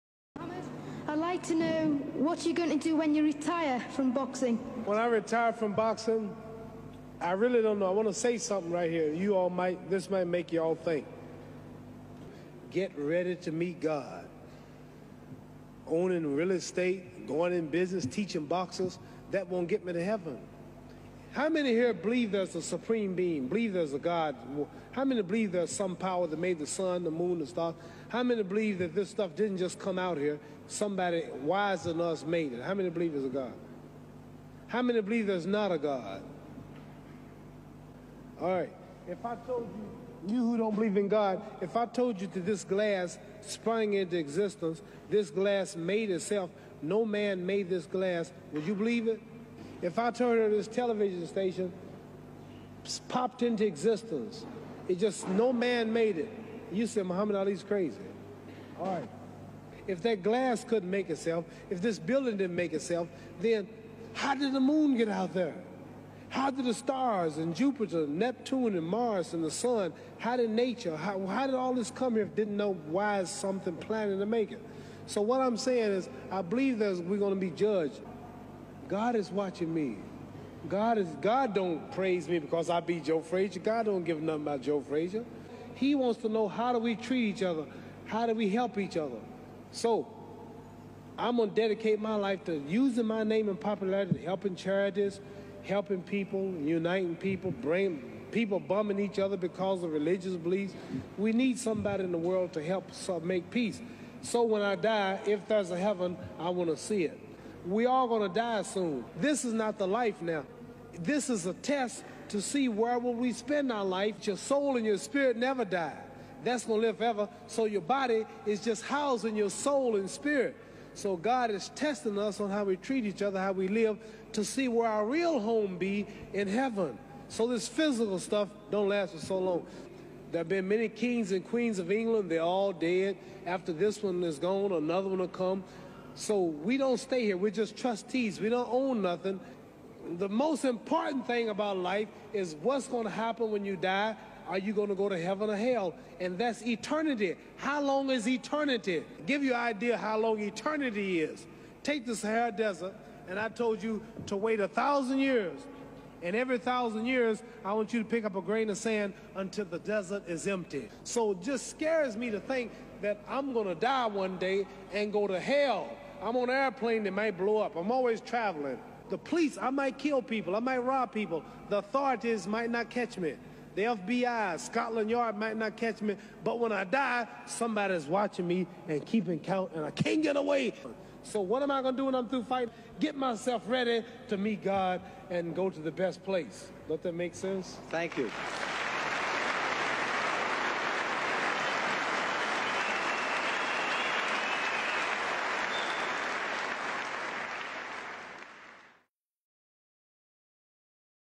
محمد علي كلاي‬ ماذا سأفعل بعد اعتزال الملاكمة؟؟ شاهد رده الرائع!!
محمد علي كلاي‬ ماذا سأفعله بعد اعتزال الملاكمة؟؟ شاهد رده الرائع!
 Mohammed Ali Clay was asked what his gonna do after boxing??